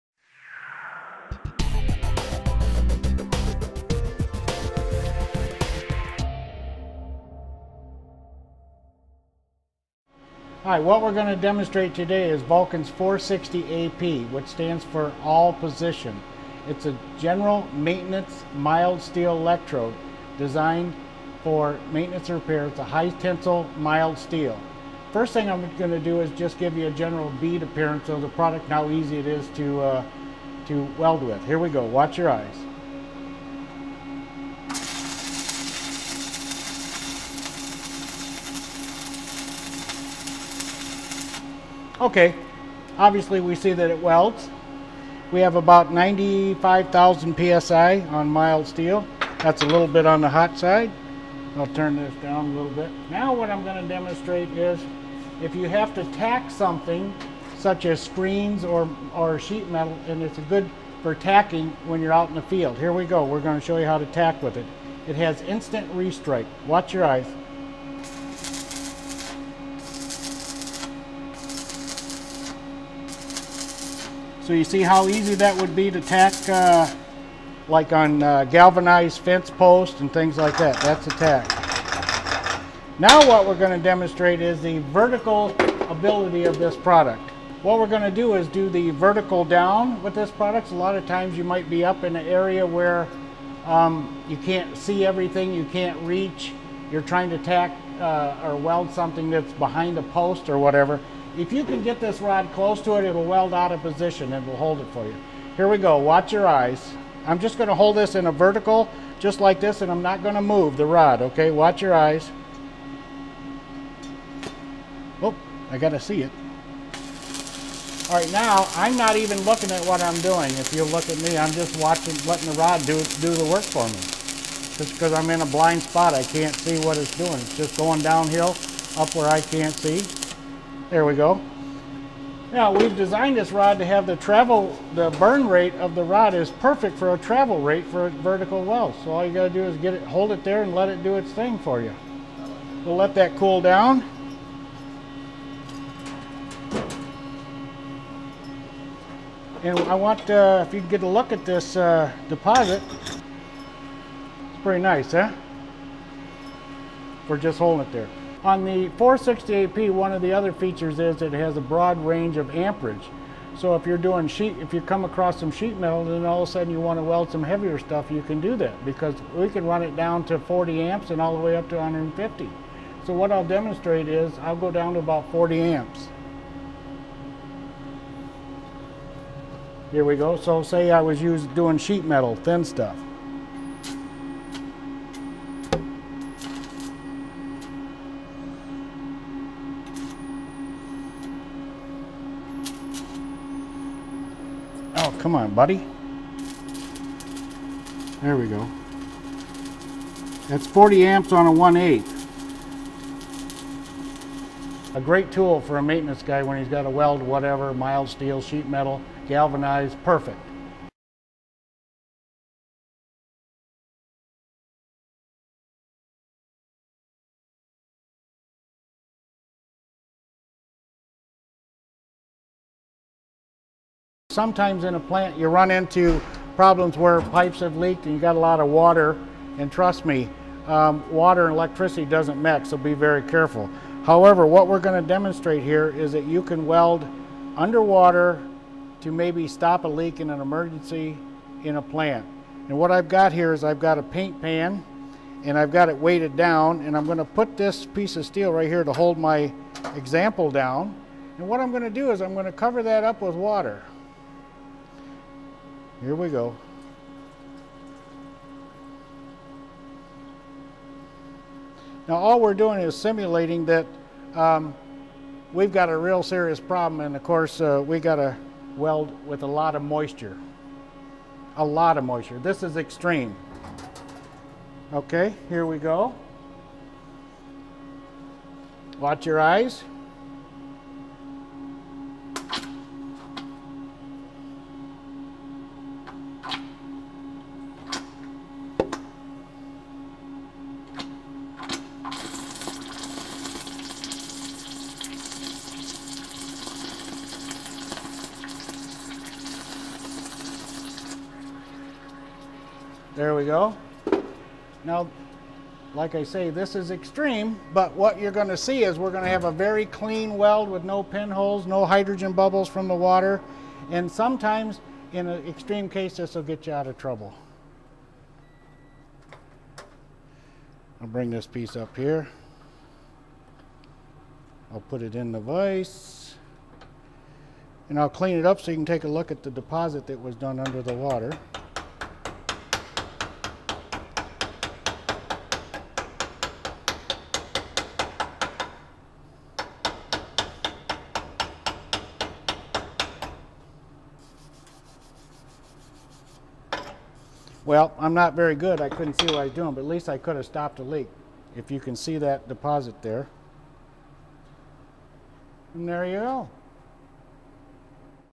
Pipe welding demo using Vulcan 460-AP - General Purpose all-position welding
A high-speed, all-position electrode specifically developed for maintenance welding of mild steels.  Operates on almost any AC or DC welding machine even with low voltage.  Its low amperage requirement controls distortion when welding thin sheet metal.  Outstanding restarting characteristics.